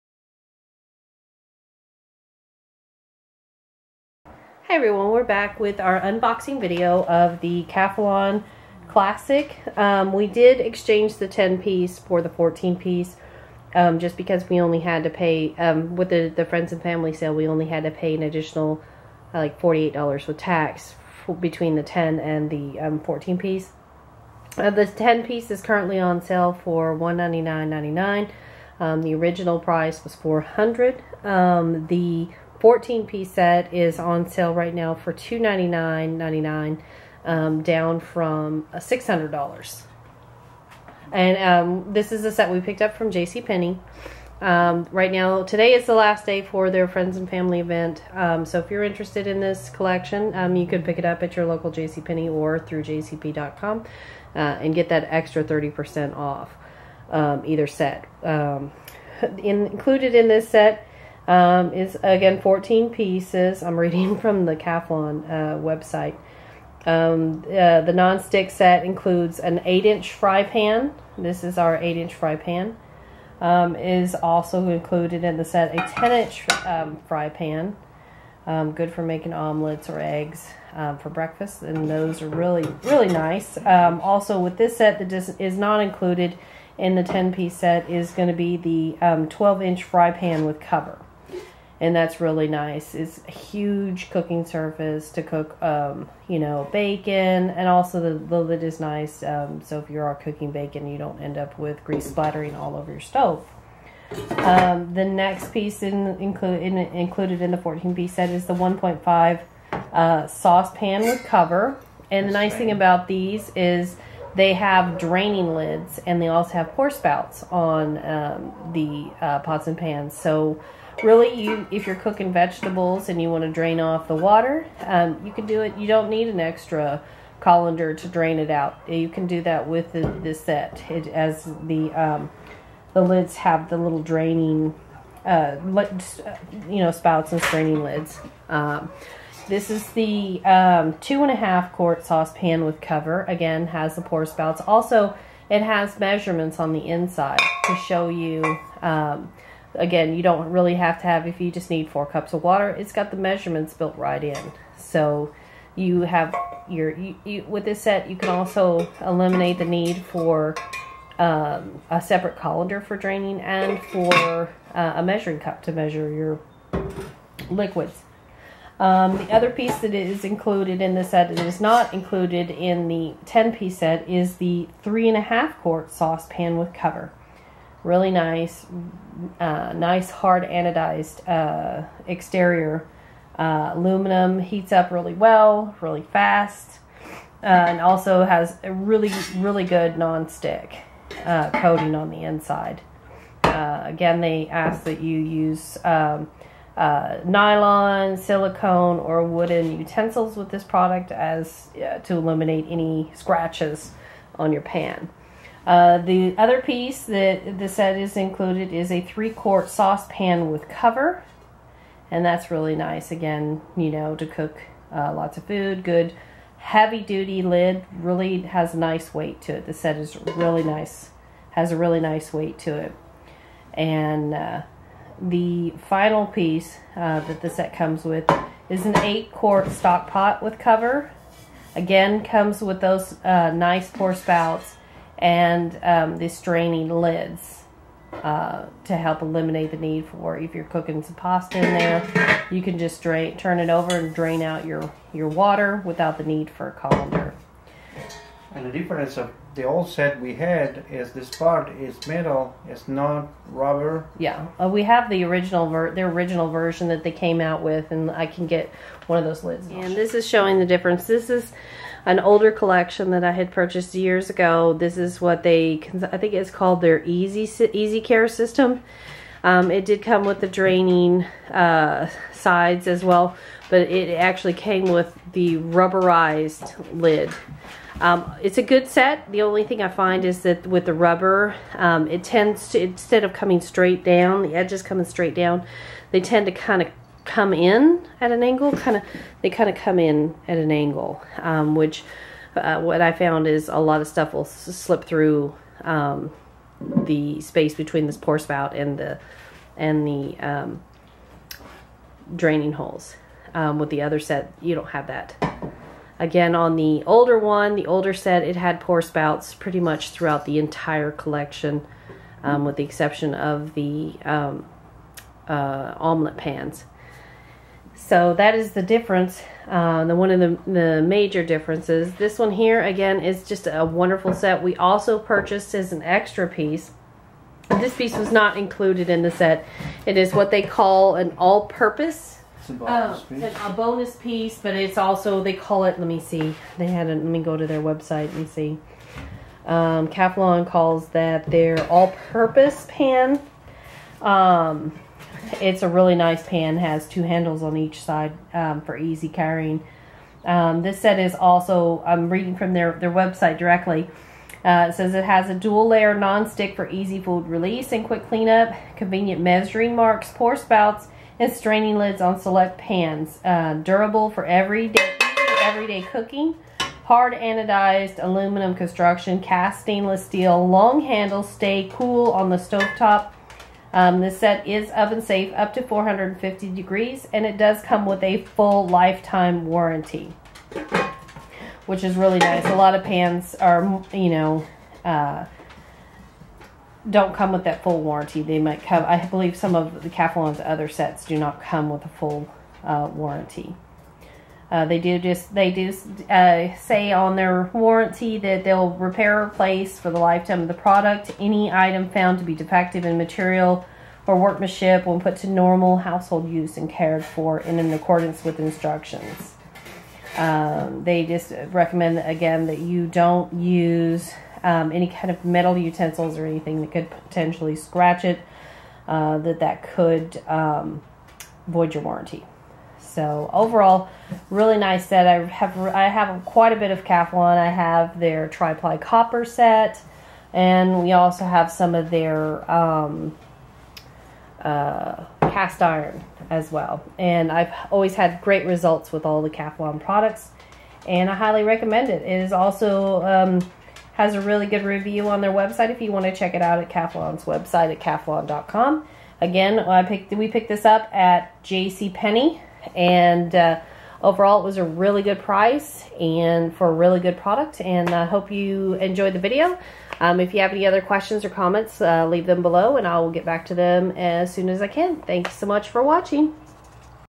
Calphalon Classic Nonstick 14pc Cookware Unboxing
This is the Calphalon Classic cookware set in the 14 pc collection. We also purchased the additional all purpose pan as well. The set retails for $299.99 on sale at JCPenney as well as other retailers.